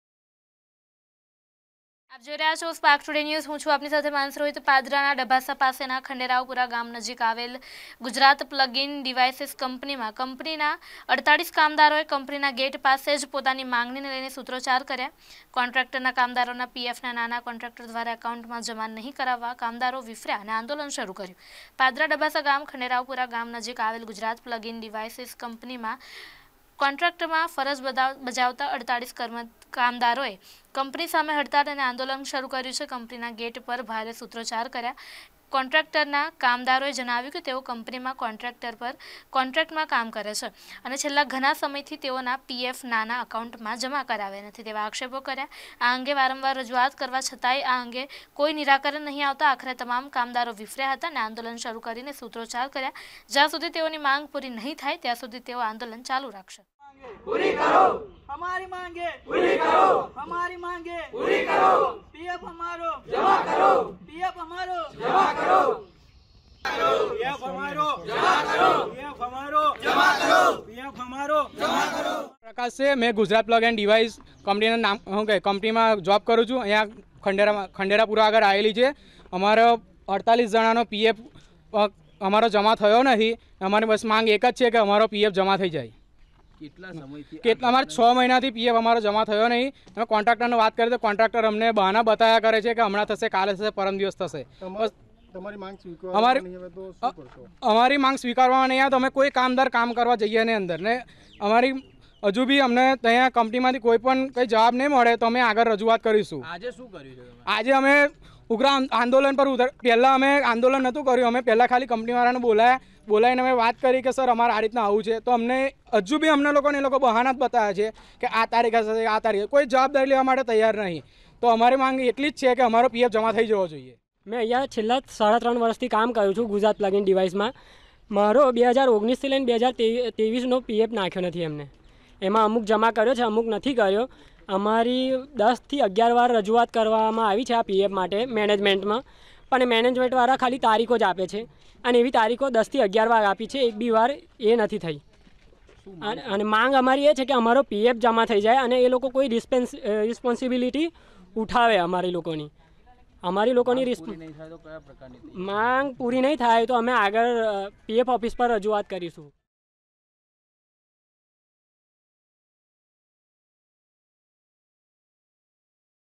પાદરાના ડભાસા પ્લગઈન ડીવાઈસીસ કંપનીના કામદારોએ માંગણીને લઈ કર્યા સુત્રોચાર કર્યા 2023 | Vadodara
પાદરાના ડભાસા પ્લગઈન ડીવાઈસીસ કંપનીના કામદારોએ  માંગણીને લઈ કર્યા સુત્રોચાર કર્યા